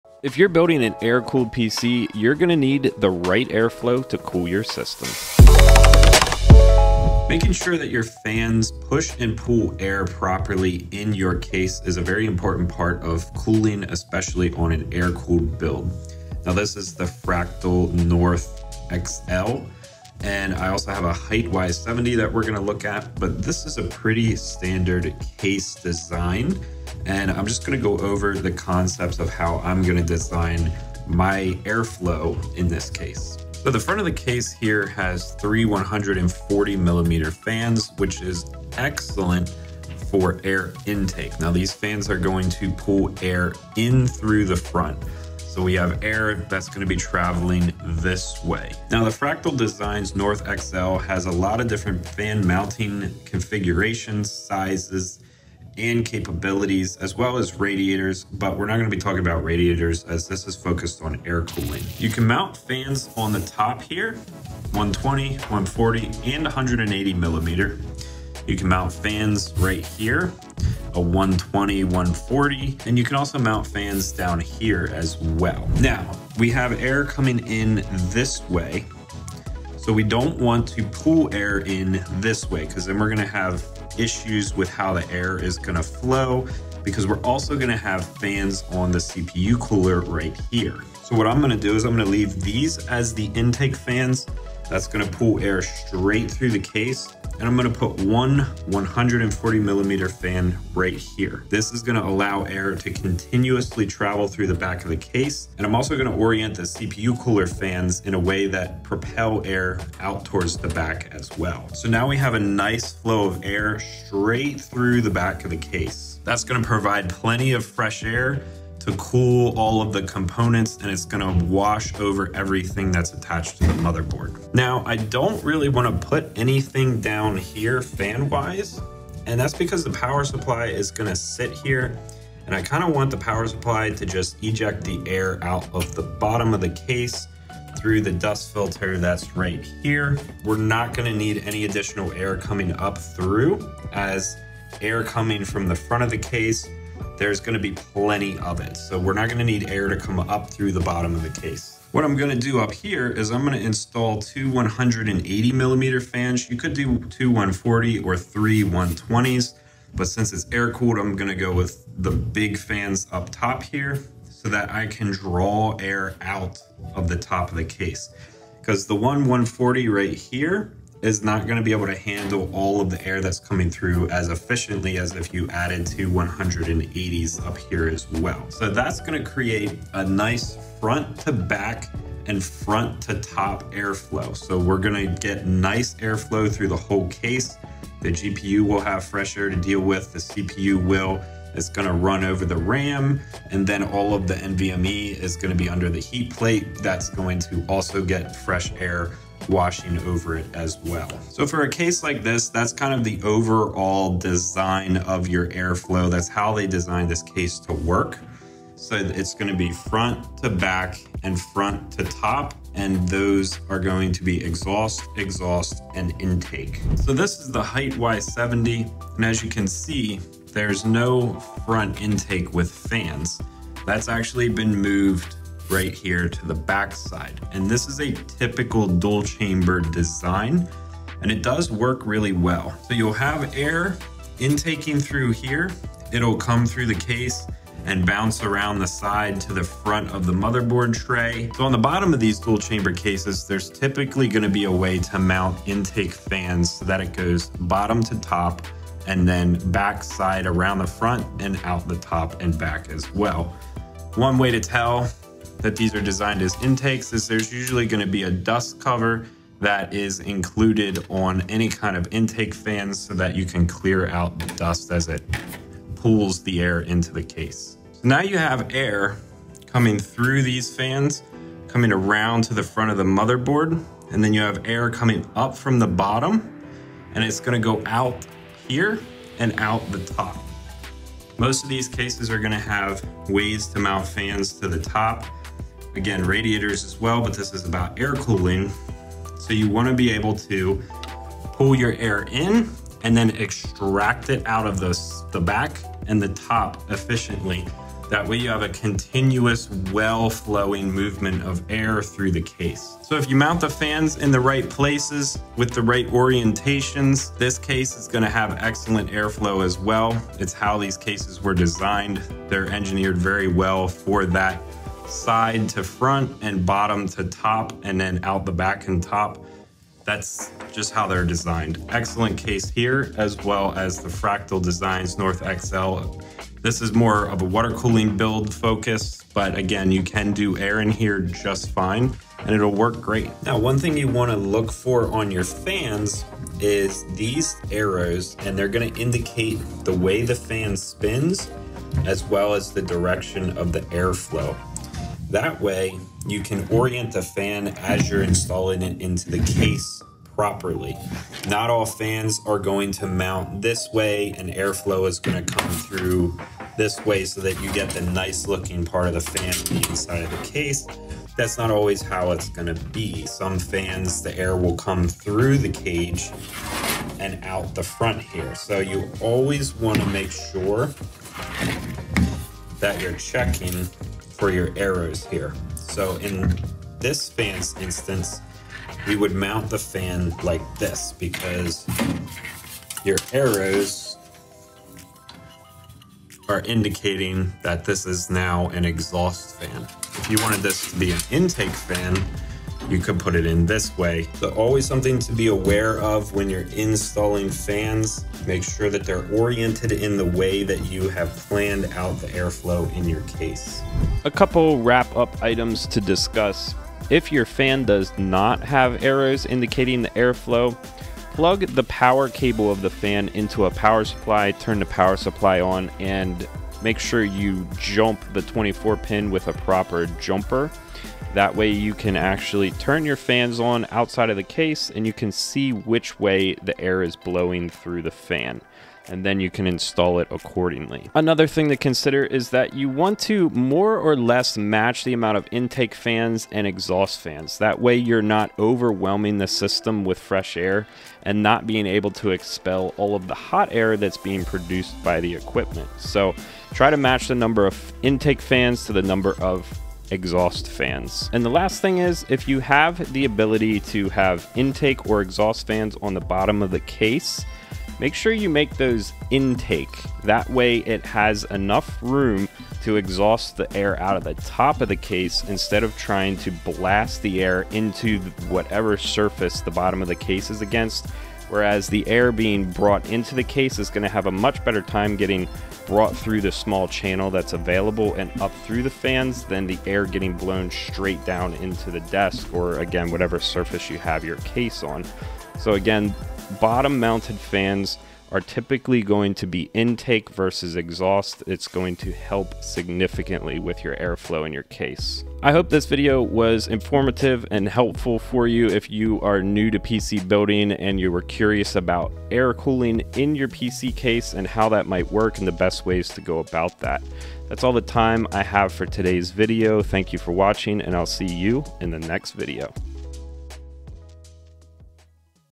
Do this to PREVENT Overheating! - Air Cooled PC Builds
Do this with your fans to prevent overheating in your air-cooled systems. Fan placement, flow direction, and distribution will have a major impact on the efficiency of your cooling system. By making sure these things all work together will ensure that your system will be able to cool even the highest performing components.